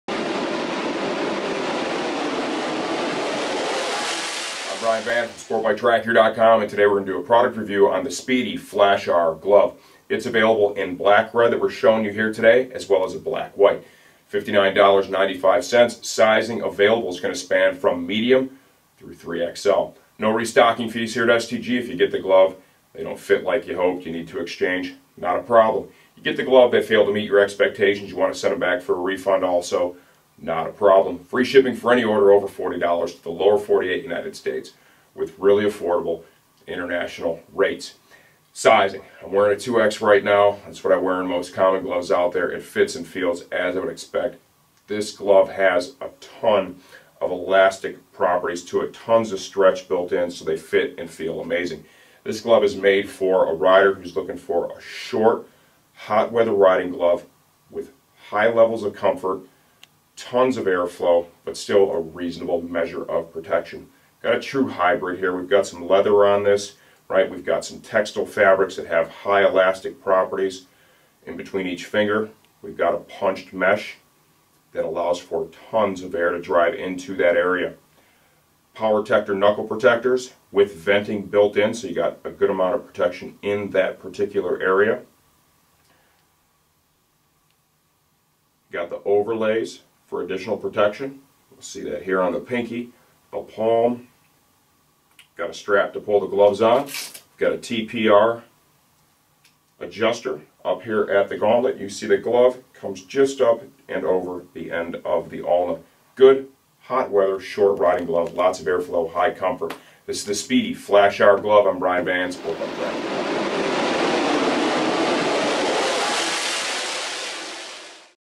Spidi Flash-R Glove Review from Sportbiketrackgear.com
Watch as Brian completes a review of the Spidi Flash-R Glove that has been a popular seller for this riding season.  This glove is price very reasonably and you get a lot of protection for daily street riding for under $60.